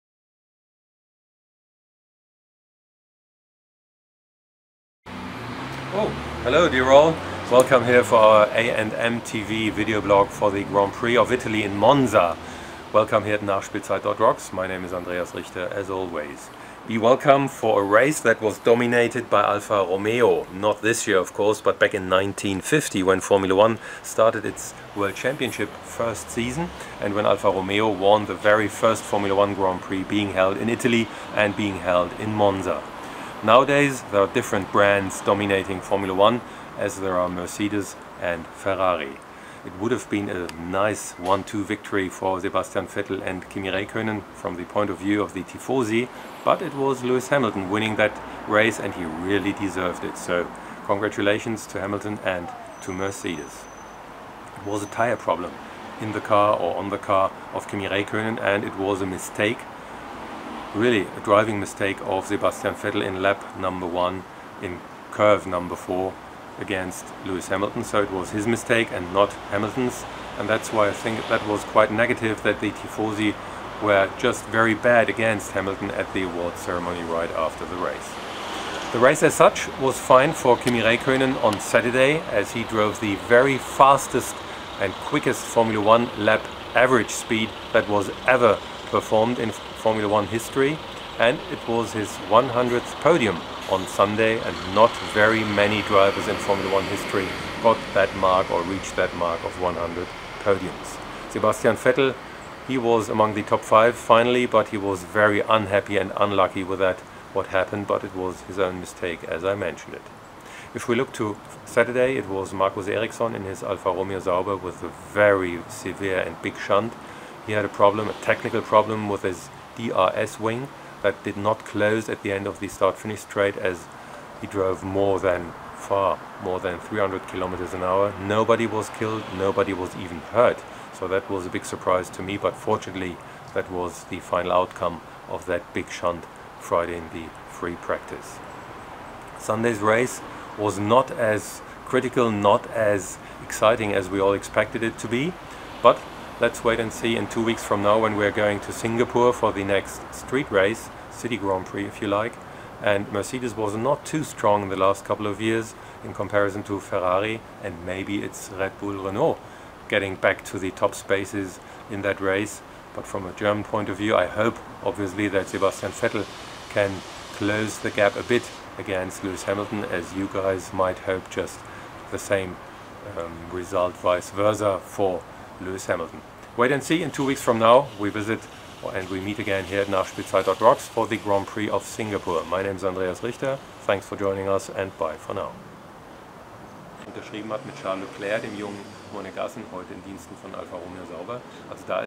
a&mTV Formula One Vlog 14 (Italy 2018)
This is the official a&mTV Formula One Videoblog of Andreas Richter, TV-Host und Motorsports expert for over 25 years. In his Videoblog he always talks about his views and opinions of the latest Grand Prix in Formula One 2018. This episode is a review on Italy 2018.